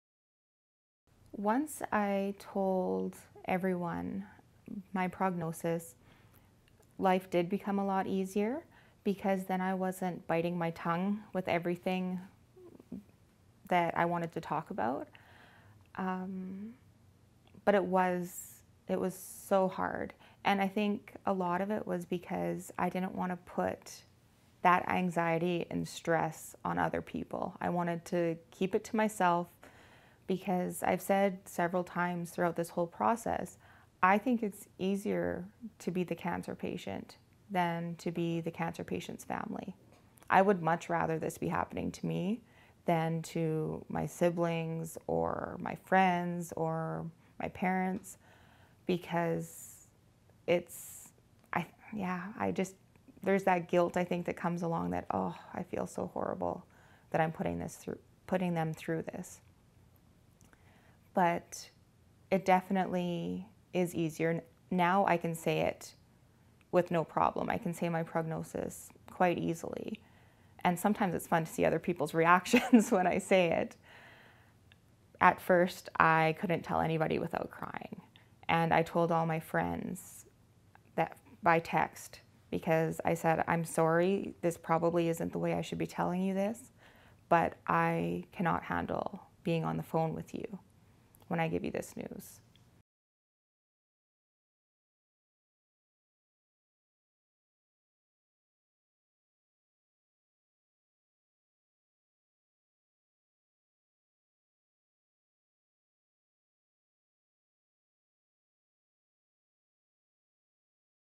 Talking about my prognosis was hard
In this video by Canadian Virtual Hospice, Janelle talks about how she negotiates talking about her prognosis with her loved ones.